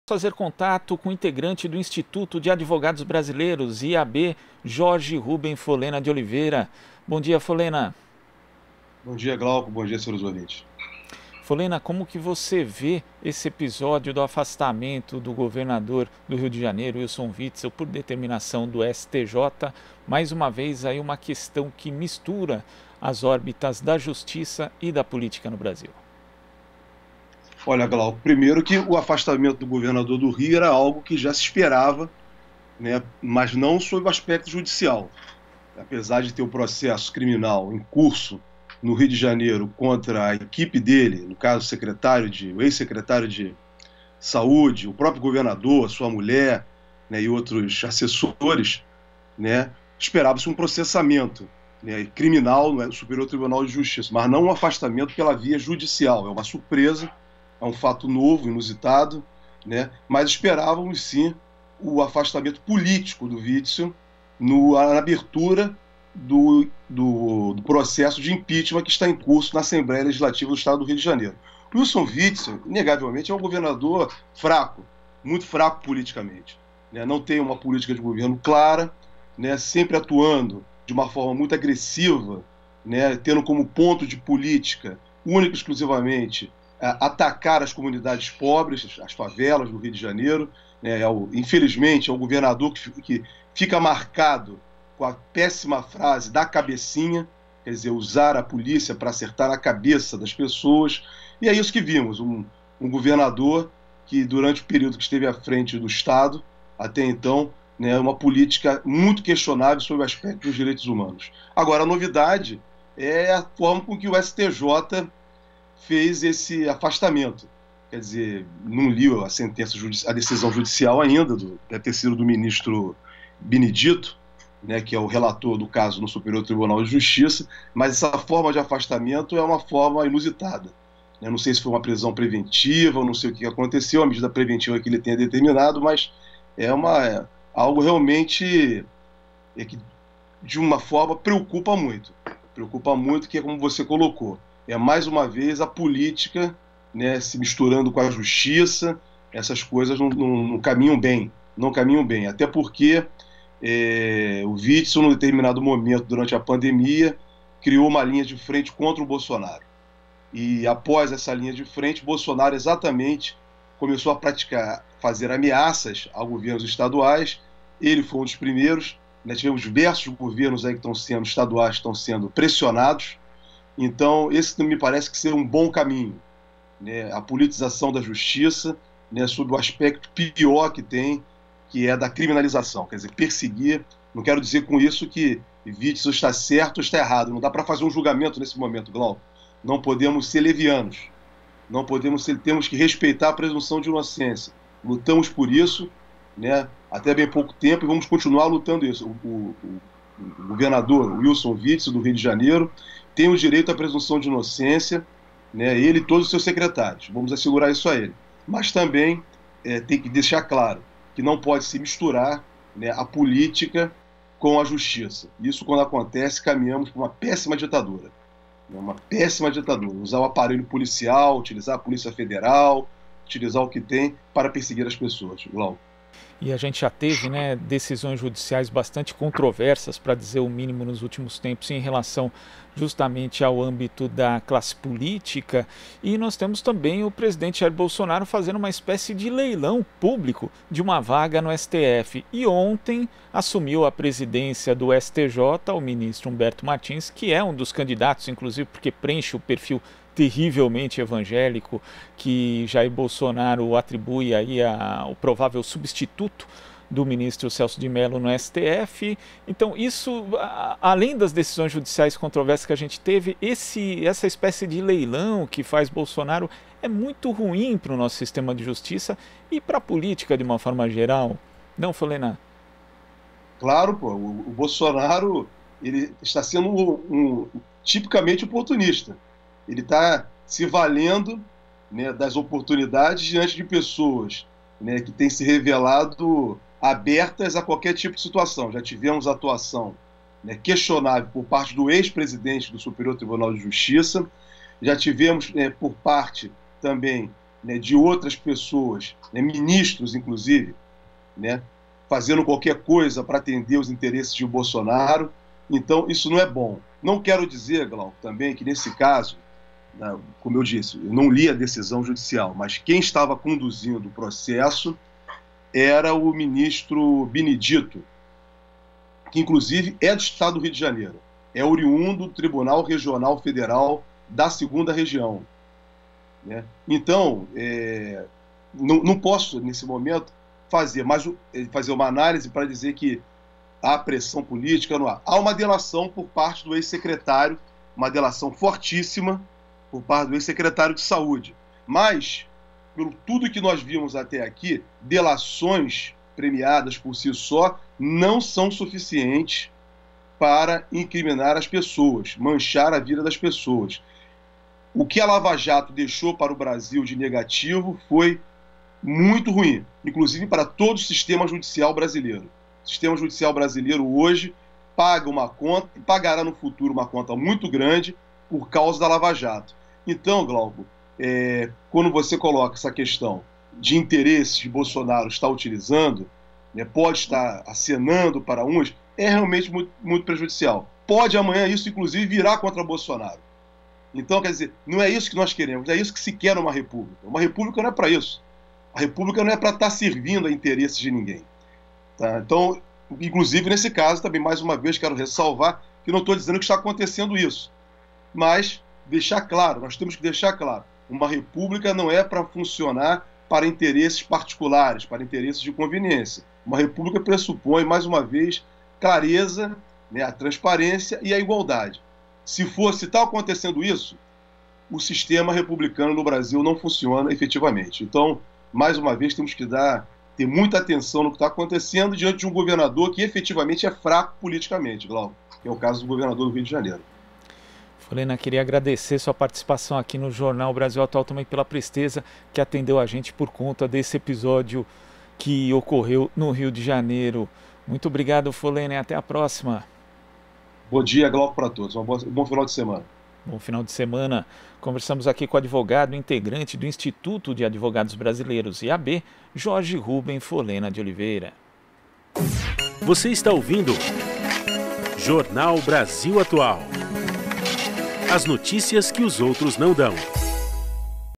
Cientista político analisa decisão da justiça de afastar Witzel do governo do RJ 🎙
O advogado e doutor em Ciência Política Jorge Rubem Folena de Oliveira, membro do Instituto dos Advogados Brasileiros, fala sobre o afastamento de Wilson Witzel do governo do Rio de Janeiro. Assista ao víde.